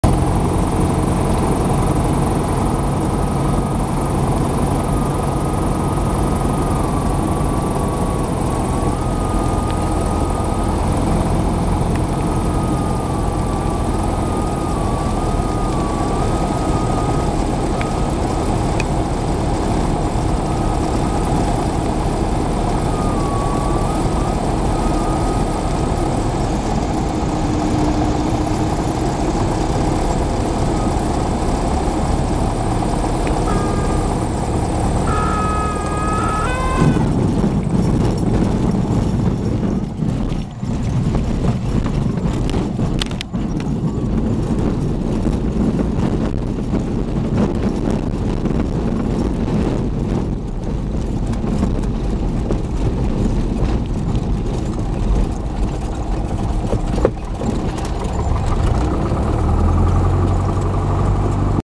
Landing at Root Ranch, Idaho
August 6 landing in a 172/180hp from Root Ranch.  We had a great breakfast here.  I'm told Root Ranch used to be a rustlers hideout, complete with slots in the barn to shoot from.   It seems this is owned by several people (It's a private strip) as there was a board mtg that day with the owners - who were also very friendly.